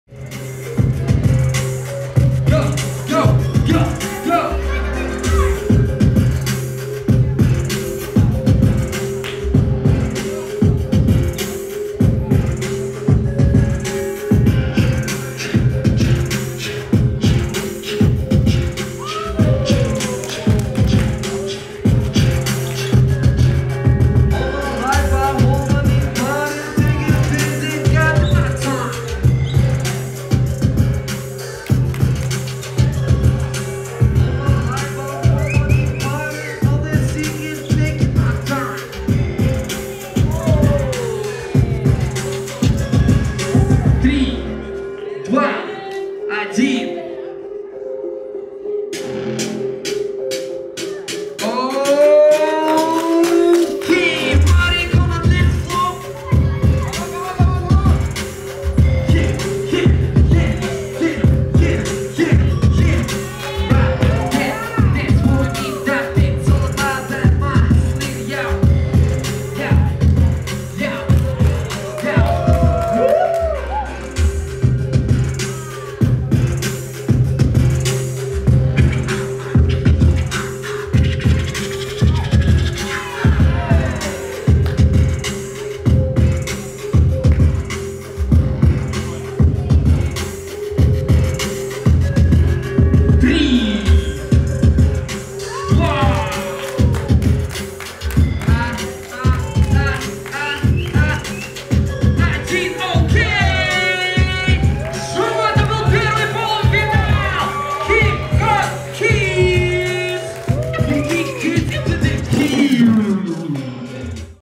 HYPE DANCE BATTLE 2017 | Hip Hop Kids 1/2 final | Dasha Turtle vs Marik
JUDGES: Zulu, Tolu, Boogs.
DJ Igorskee.
MC Armo.
24.12.17 | Kyiv, Ukraine